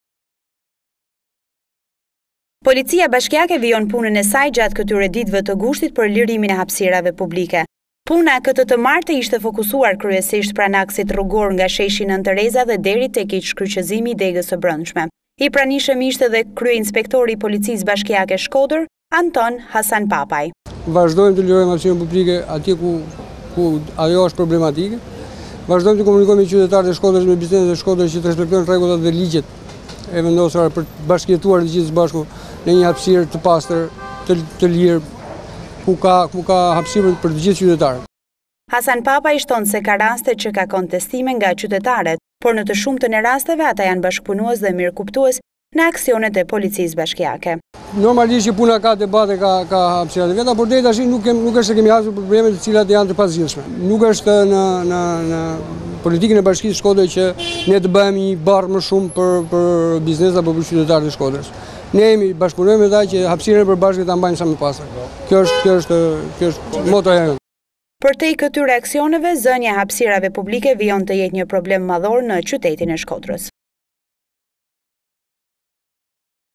“Aksion” për lirimin e hapësirave publike - Antena TV - News
POLICIA BASHKIAKE VIJON PUNEN NORMALISHT DHE GJATË MUAJIT GUSHT PËR LIRIMIN E HAPSIRAVE PUBLIKE.
KËTË TË MARË PUNA E POLICISË BASHKIAKE ISHTE FOKUSUAR NË QËNDER TË QYTETIT, KU U BËNË TË MUNDURA DISA LIRIME TROTUARESH TË ZËNA NGA BIZNESE PRIVATE